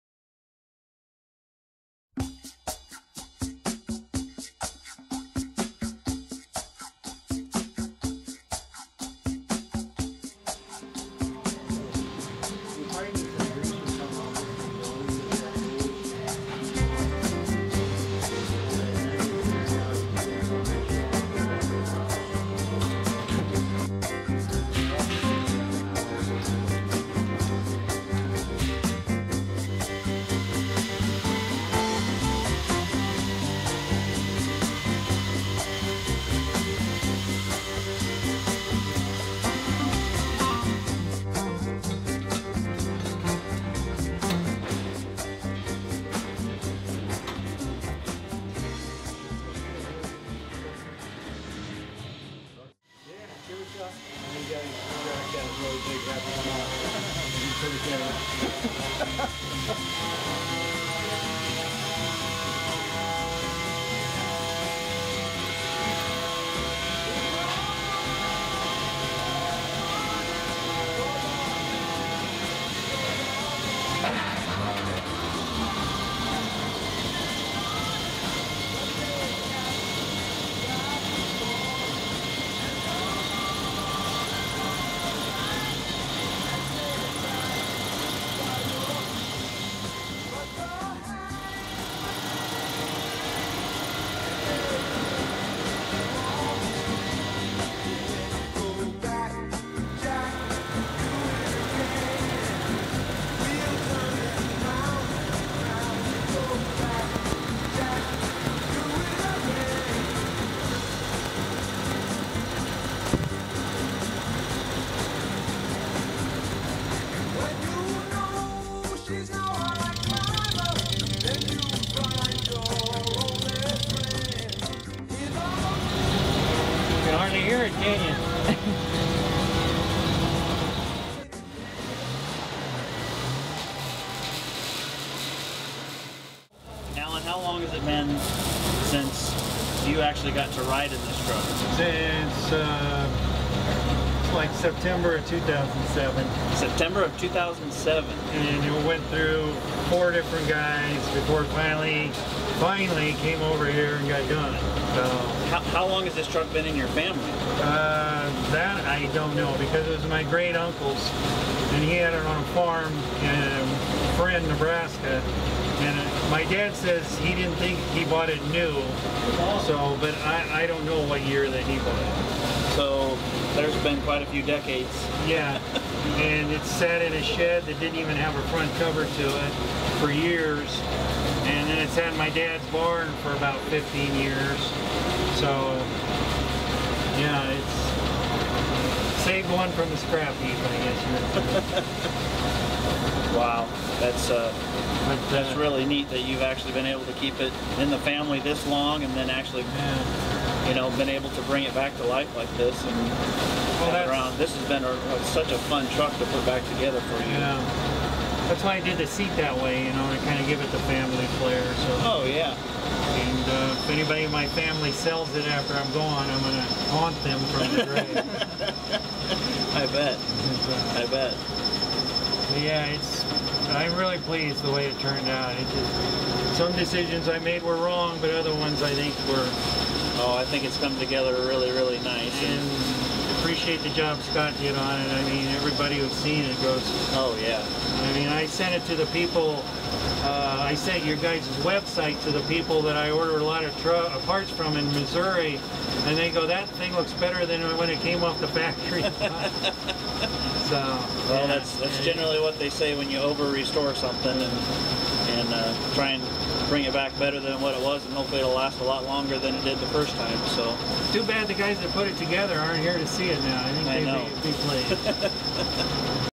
1935 Chevy Pickup Restoration by RAMC
1935 Chevy Pickup body work, paint, bed assembly, interior assembly, engine work, etc. by RAMC.